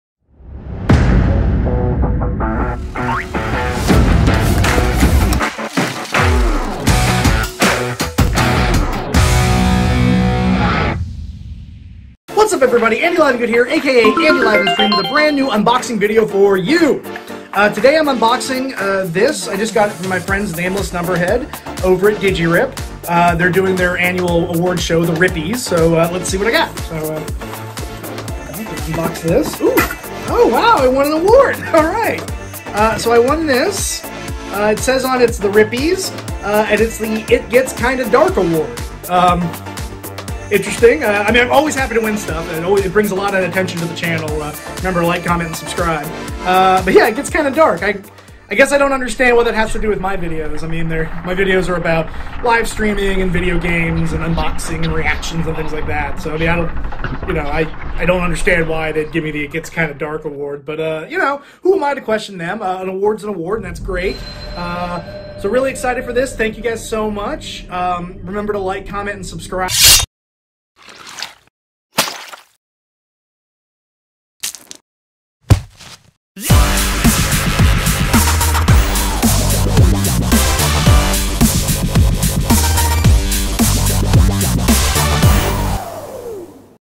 Andy LivenSTREAM Episode 8- Awards Show
Andy LivenSTREAM wins the "It Keeps Getting Darker" award during the 2021 Rippies Awards. (Put on by Nameless Numberhead and Digi Rip)

For more: AndyLivengood.com